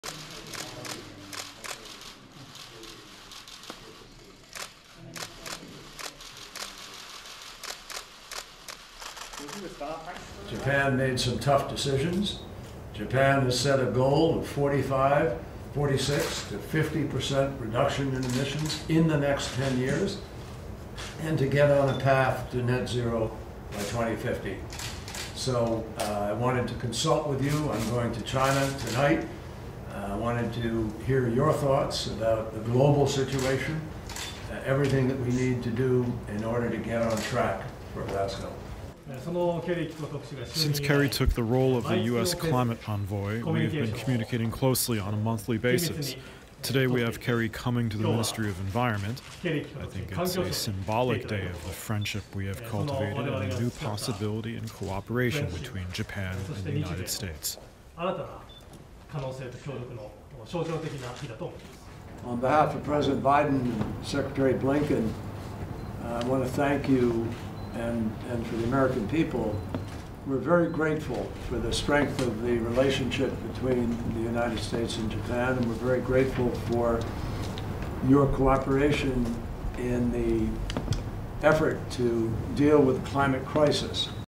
John Kerry visits Japan to discuss cutting emissions
U.S. climate envoy John Kerry held talks in Tokyo on Tuesday with Japan's Prime Minister Yoshihide Suga and other ministers on cooperation on carbon emissions and cutting support for fossil fuels.